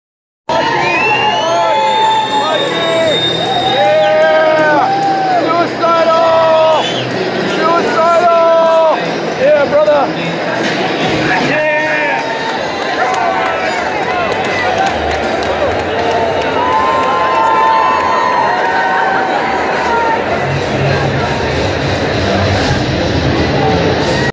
Mike Muir of Suicidal Tendencies greeting the fans after opening for Metallica Glendale 9.9.2023
Phoenix got lucky with the addition of S.T. to an already stacked lineup. Suicidal sounds as killer as ever!